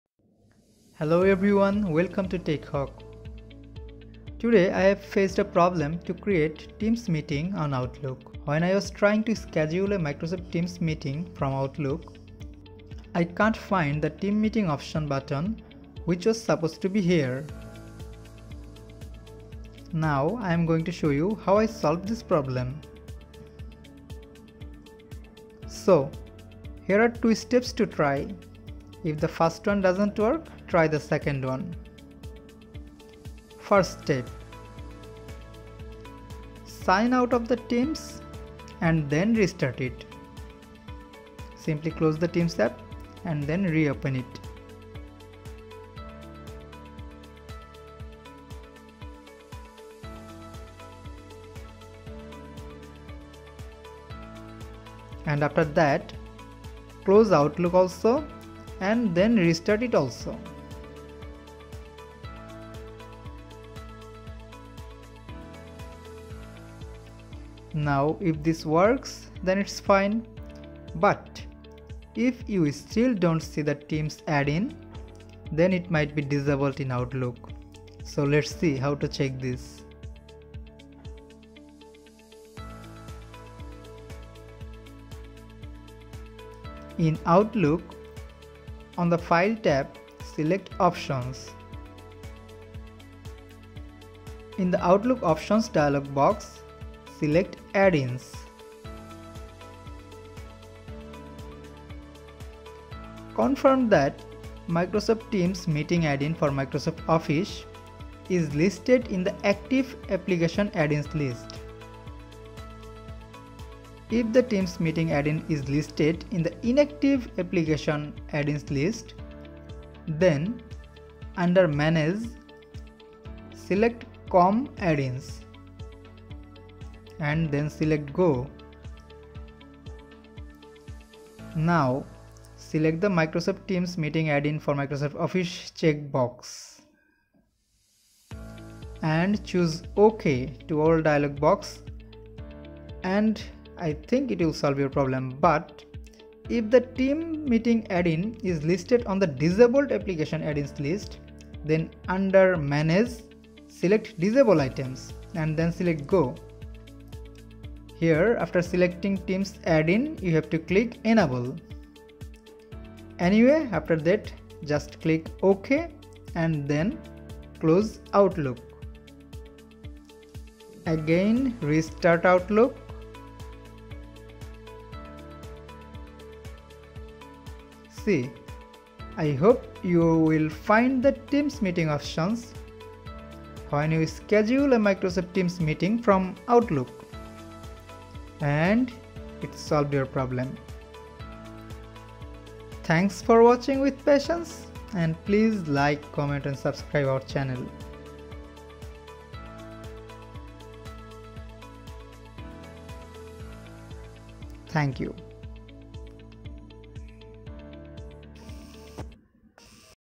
Solve: Teams Meeting Button Not Showing Up in Outlook | Teams Meeting Option Missing from Outlook
Hello everyone. Welcome to Tech Hawk.
Today I have faced a problem to create teams meeting on outlook. When I was trying to schedule a ‘Microsoft Teams’ meeting from Outlook, I can’t find the Team meeting option button which was supposed to be here. Now I am going to show you how I solved this problem.